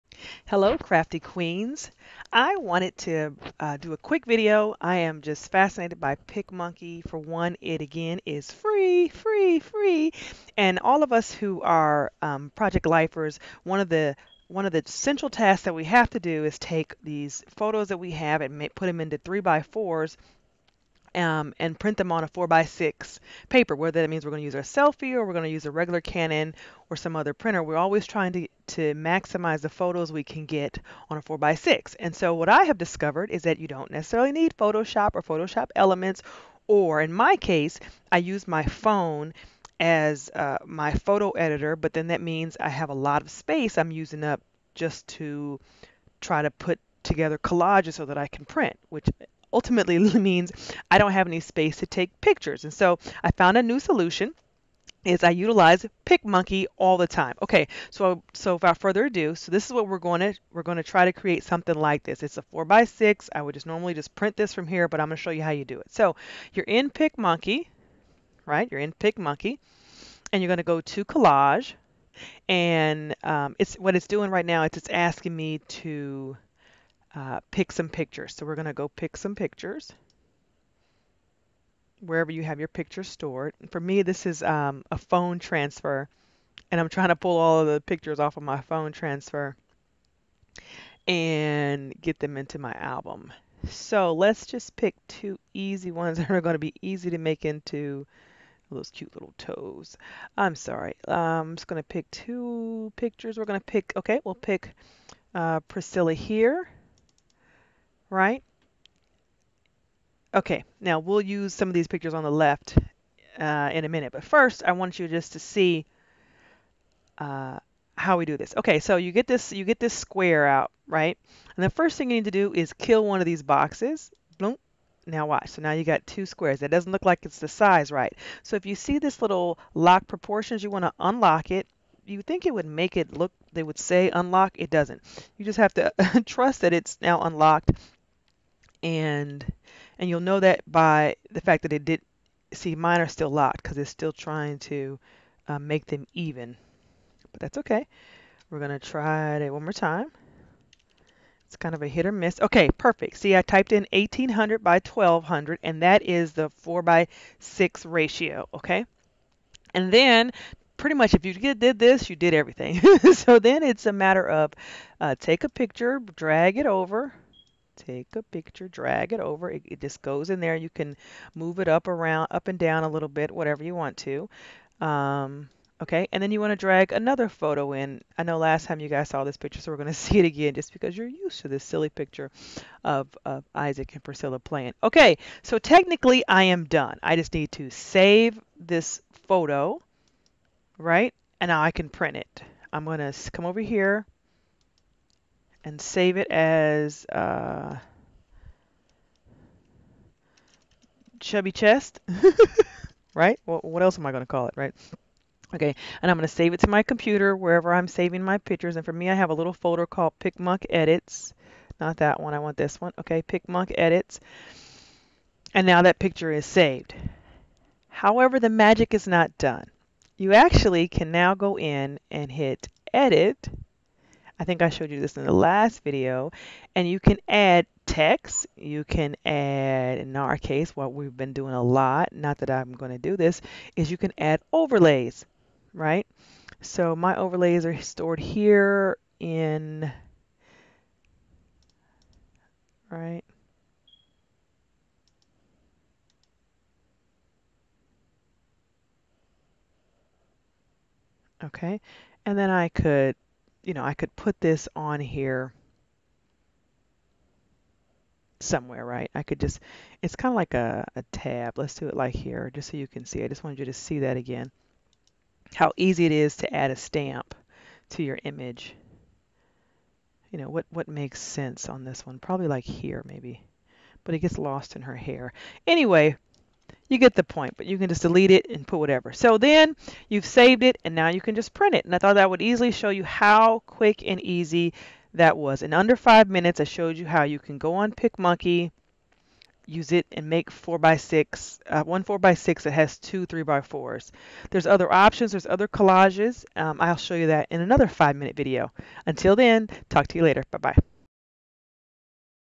Printing 3x4 photos using Pic Monkey
How to print 3x4 photos on a 4x6 photo using free application Pic Monkey